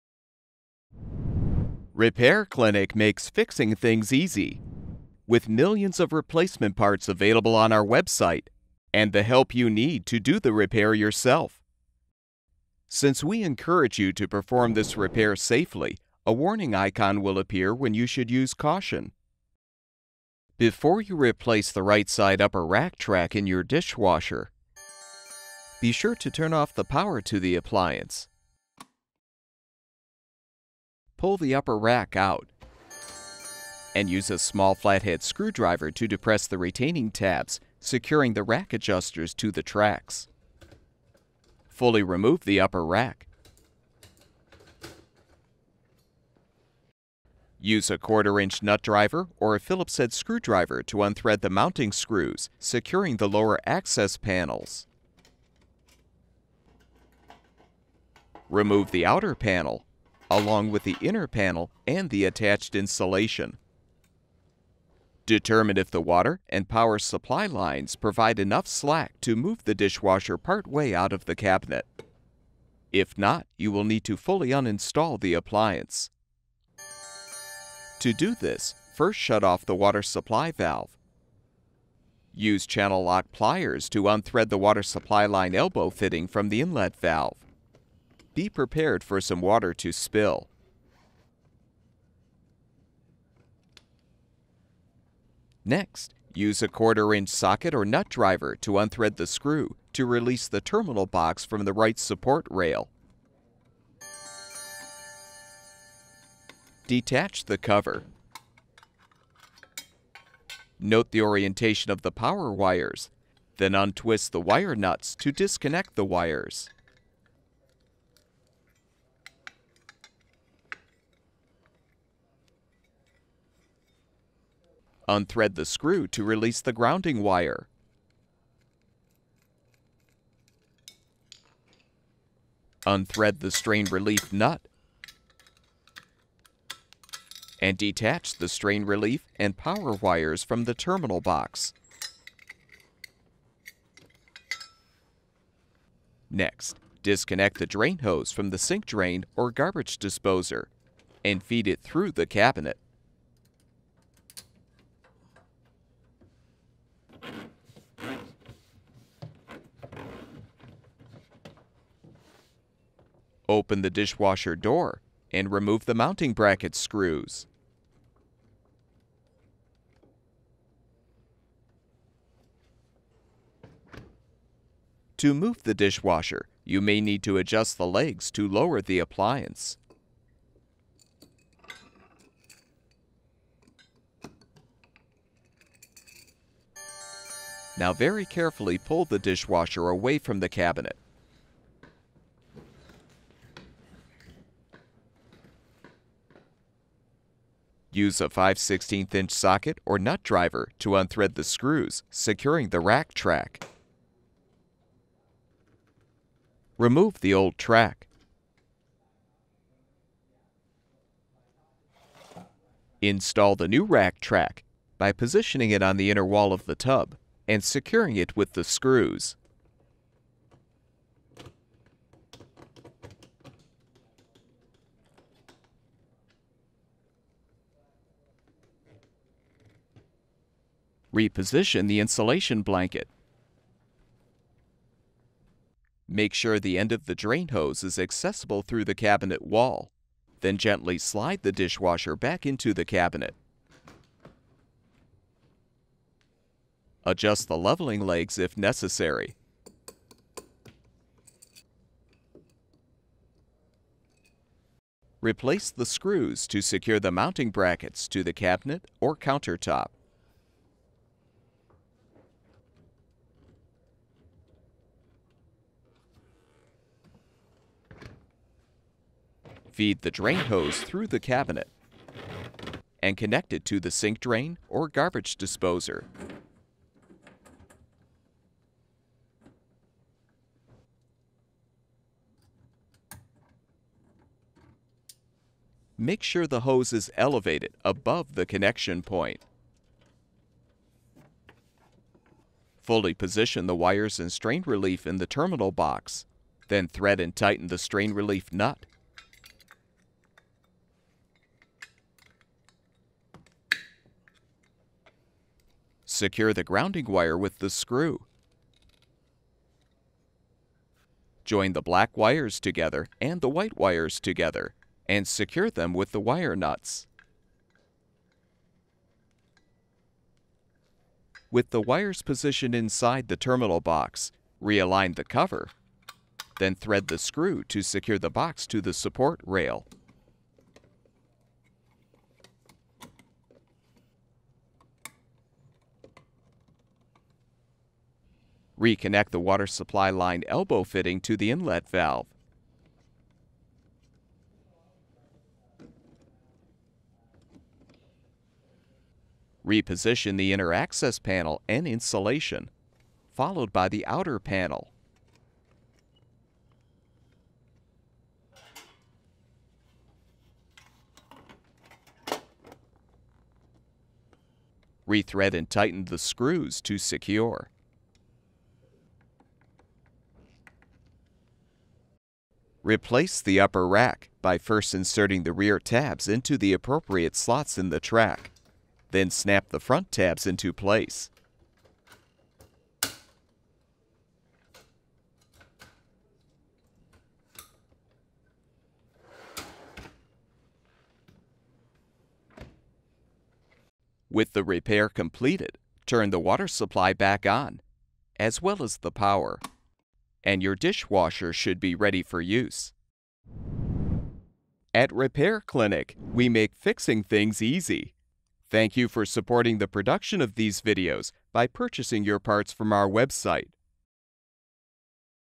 KitchenAid Dishwasher Right Upper Rack Track
This video provides step-by-step repair instructions for replacing the right upper rack track on a KitchenAid dishwasher. The most common reason for replacing the track is if the rack will not move smoothly.

All of the information for this rack track replacement video is applicable to the following brands:
Kenmore, KitchenAid, Whirlpool, Ikea, Amana, Jenn Air, Maytag

Tools needed: 1/4" nut driver, Channel Lock pliers, flat head screwdriver